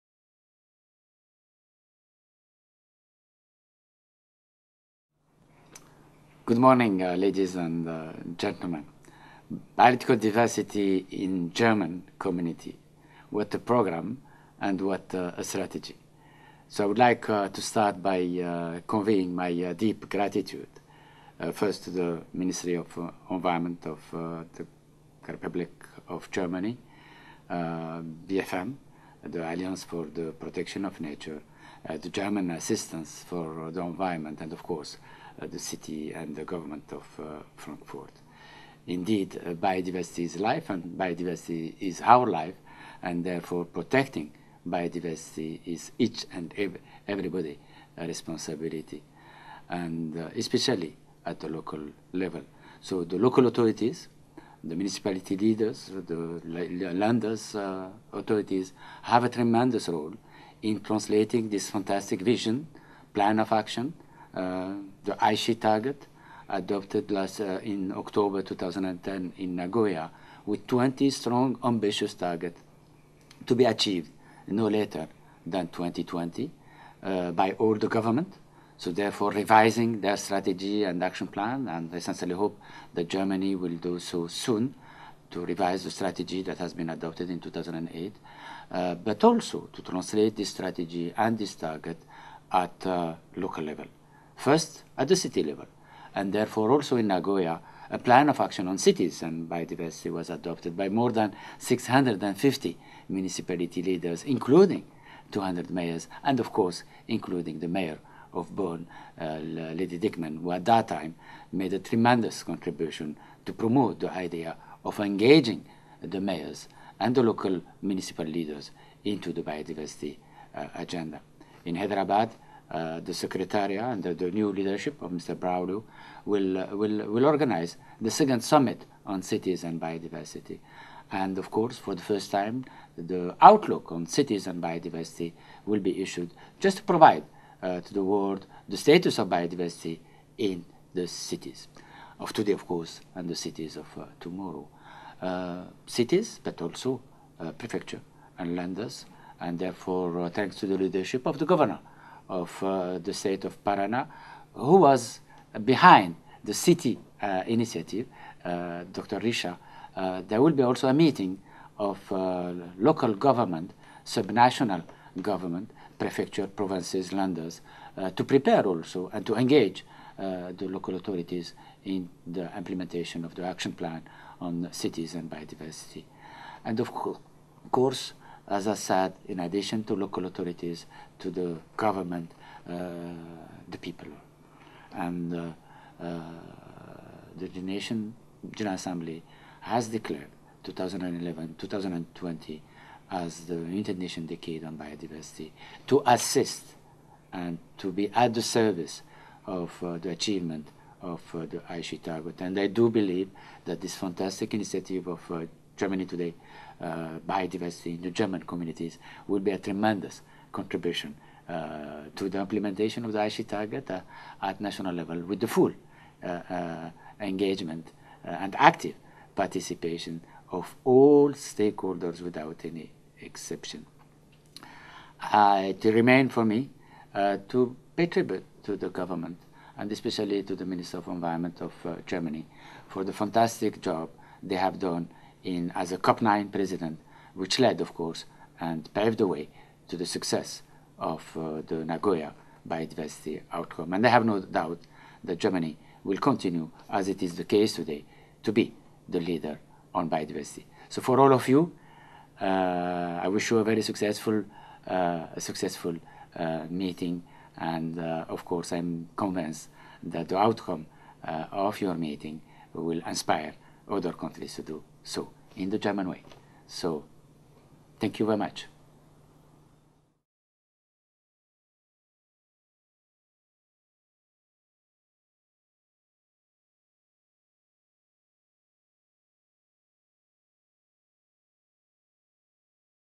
Video message of Mr. Ahmed Djoghlaf, former Executive Secretary of the CBD, February 2, 2012
Video message of Mr. Ahmed Djoghlaf, former Executive Secretary of the CBD, at the occasion of the launch of a German Alliance of Municipalities, February 2, 2012